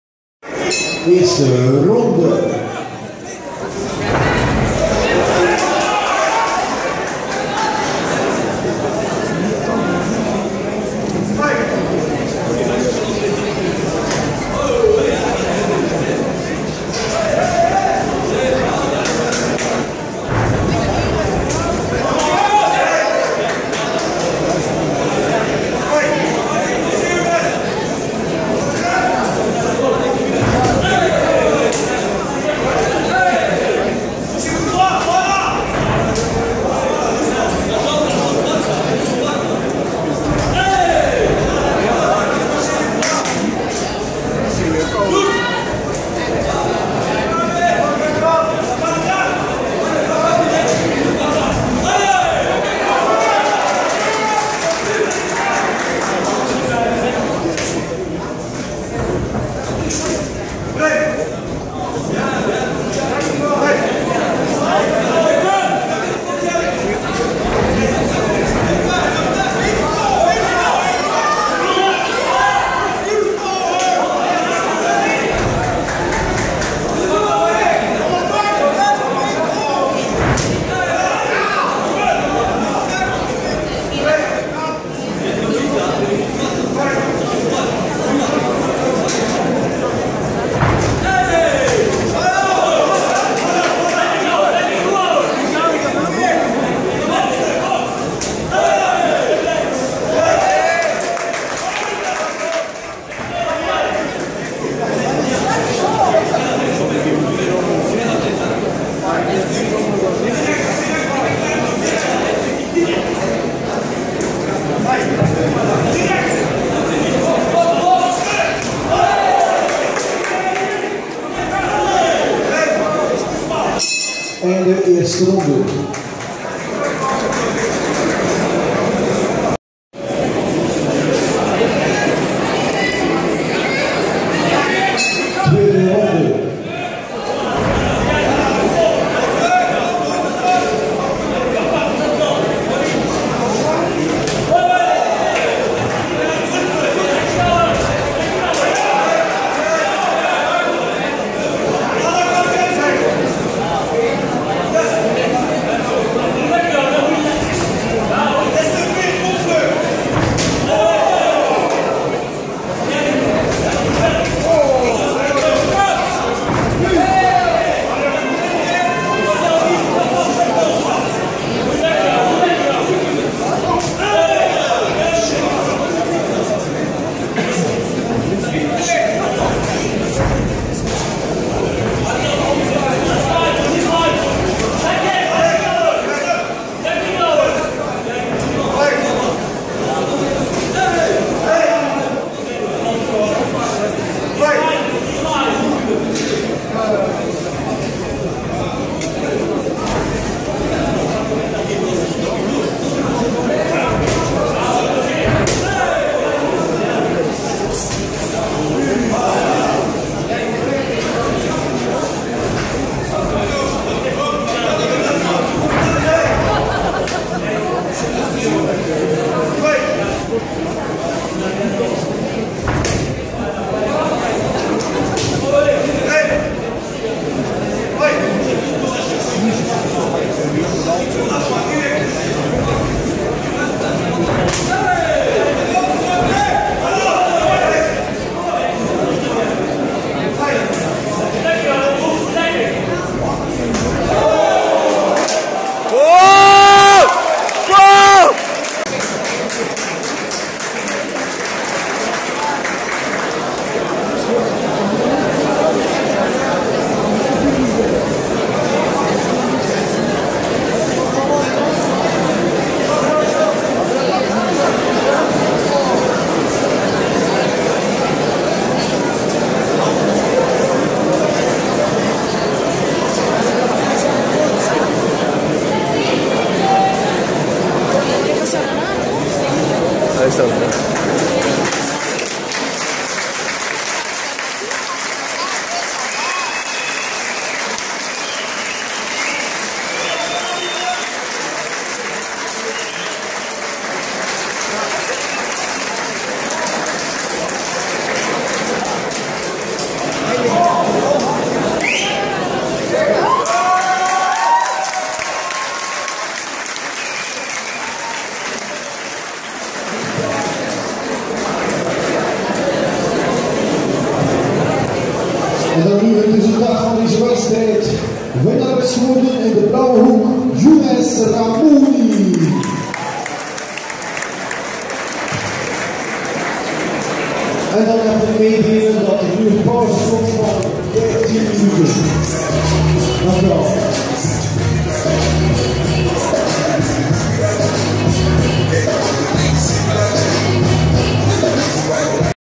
Younes Lek XTC TEAM AISSA vs Gokhan Pehlivan GOLDEN GLOVES
Classe B // 5x2 min
06/12/2014
Fight Night
Gent